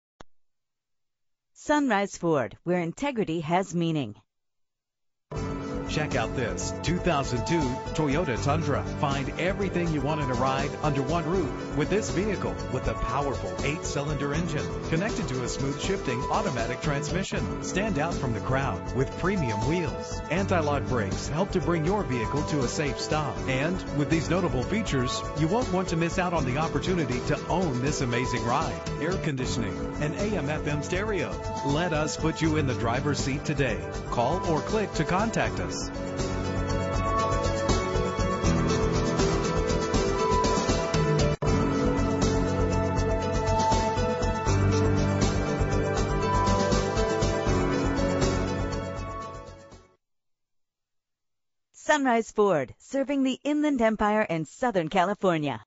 2002 TOYOTA TUNDRA - Sunrise Ford Fontana CA
Year: 2002
Make: TOYOTA
Model: TUNDRA
Trim: LTD V8
Engine: 4.7 liter 8 cylinder dohc
Transmission:
Color: black
Mileage: 86913
Address: